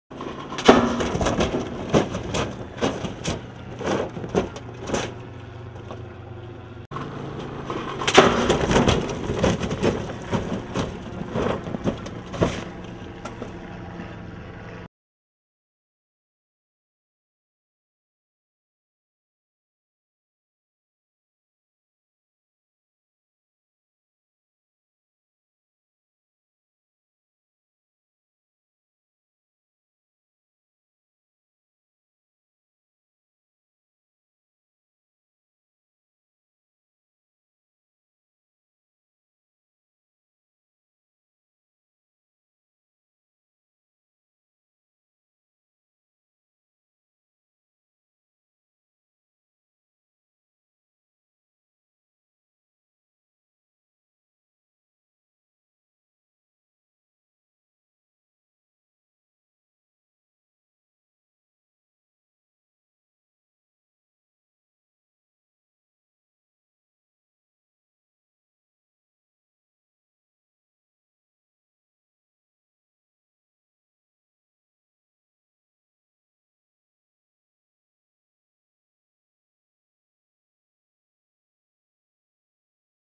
1998 Ford Expedition High-Speed Sled Rollover Crash Test
This video shows the devastating effects of what happens when you don't wear your seat belts and your vehicle leaves the motorway. This is a prime example how seat belts can save someone's life, or even lives. ALWAYS buckle up and drive safely